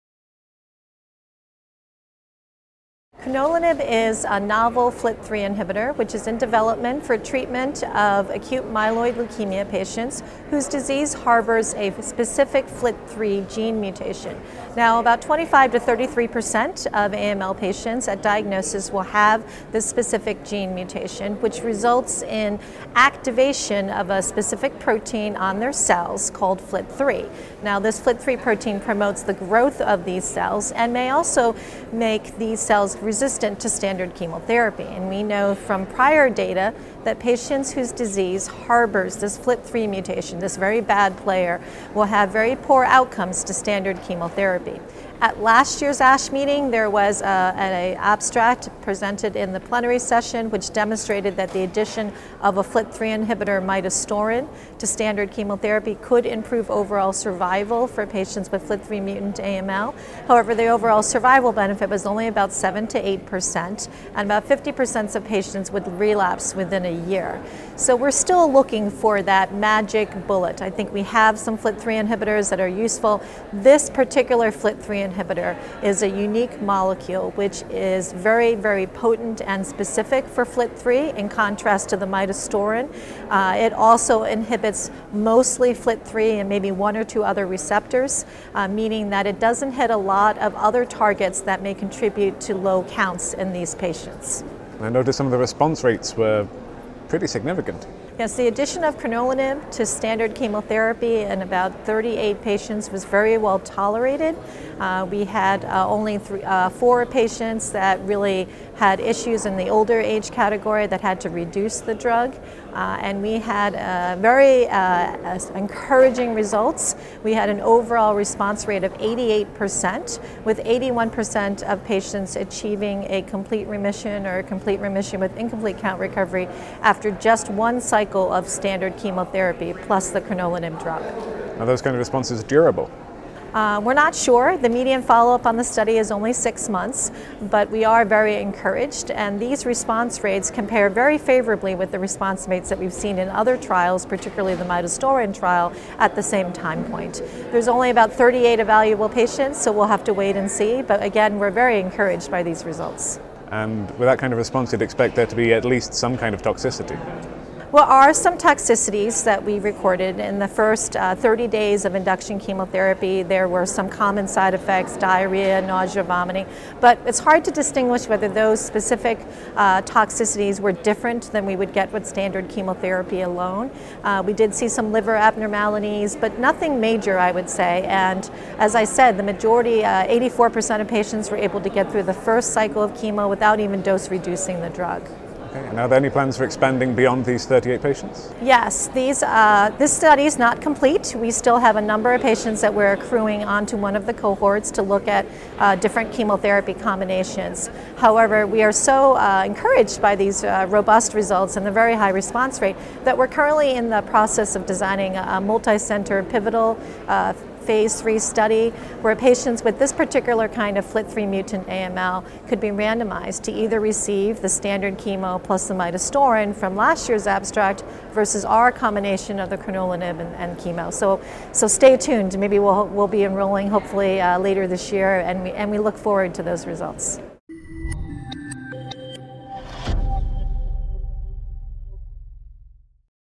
How can we use crenolanib to treat AML?
Dr Wang meets with ecancer at ASH 2016 to discuss crenolanib, a type I FLT3 TKI inhibitor, that can be safely combined with cytarabine and anthracycline induction chemotherapy.

This results in high response rates in patients with newly diagnosed FLT3 mutant acute myeloid leukaemia (AML).